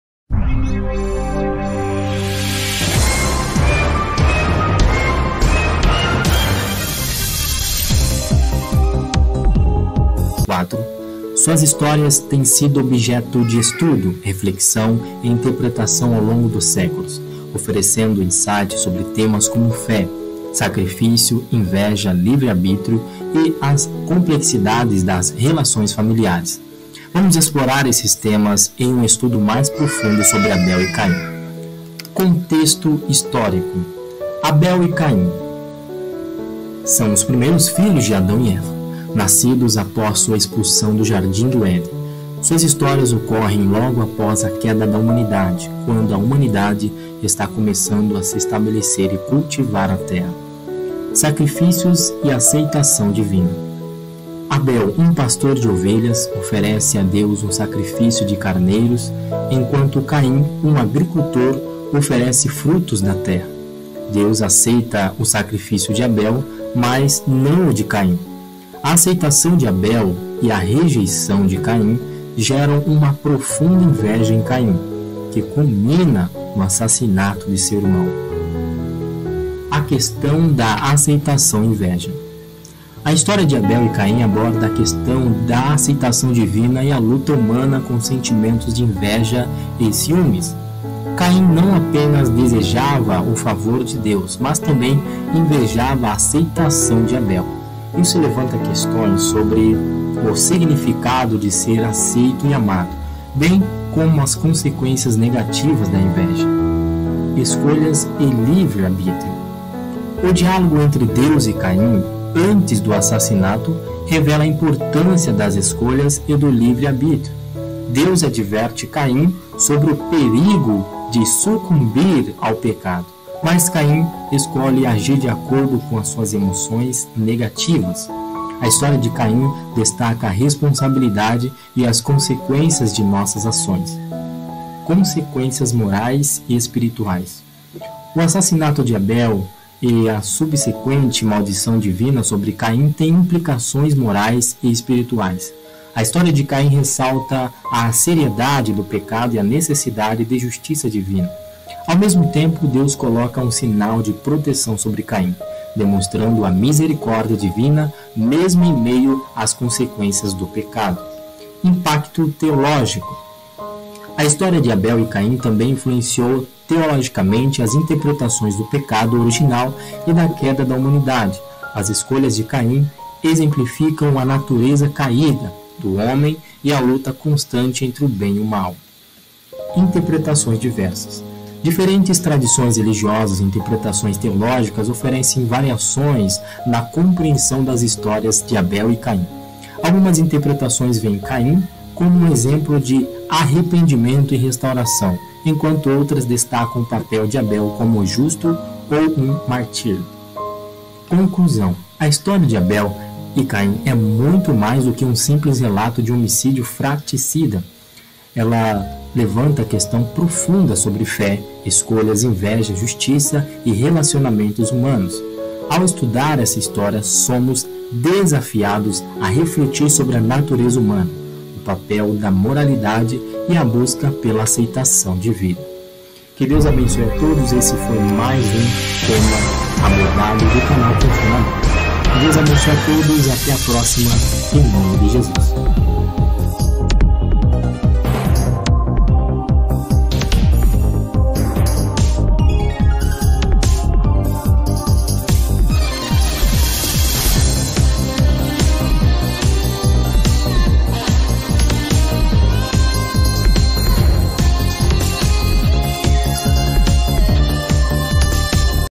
ABEL E CAIM - PERSONAGENS DE GÊNESIS - QUEM FOI NA BIBLIA
Abel e Caim: Explorando Suas Histórias e Significados Profundos

Abel e Caim são figuras bíblicas apresentadas na narrativa de Gênesis, capítulo 4. Suas histórias têm sido objeto de estudo, reflexão e interpretação ao longo dos séculos, oferecendo insights sobre temas como fé, sacrifício, inveja, livre arbítrio e as complexidades das relações familiares. Vamos explorar esses temas em um estudo mais profundo sobre Abel e Caim.

Contexto Histórico:
Abel e Caim são os primeiros filhos de Adão e Eva, nascidos após sua expulsão do Jardim do Éden. Suas histórias ocorrem logo após a queda da humanidade, quando a humanidade está começando a se estabelecer e cultivar a terra.